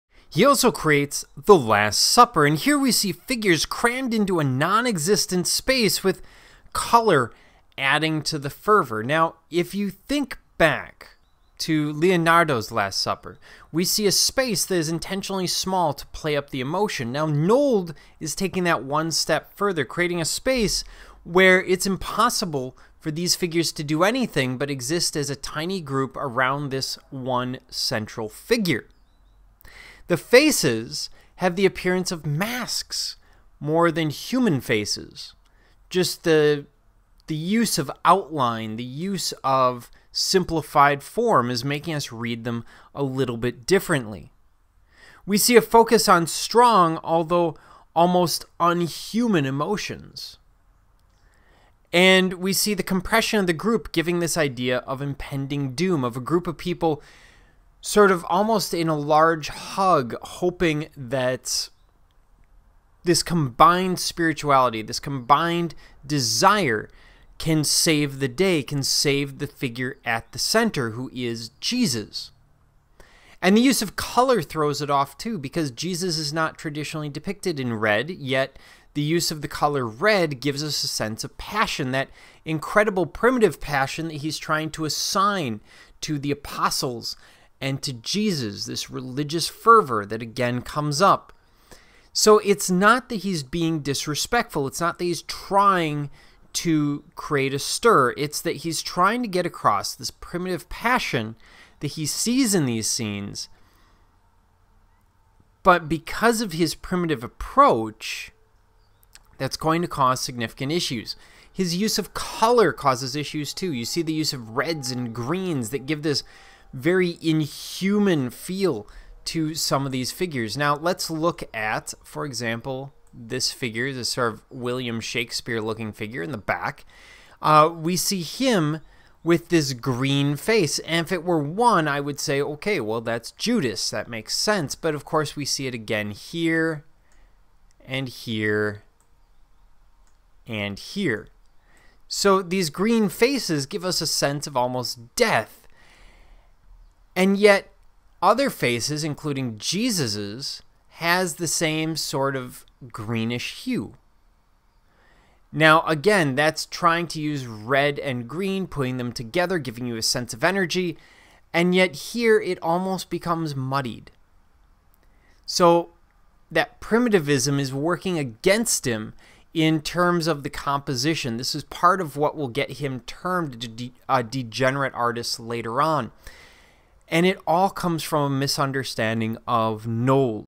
Nolde The Last Supper
A brief discussion of Nolde's The Last Supper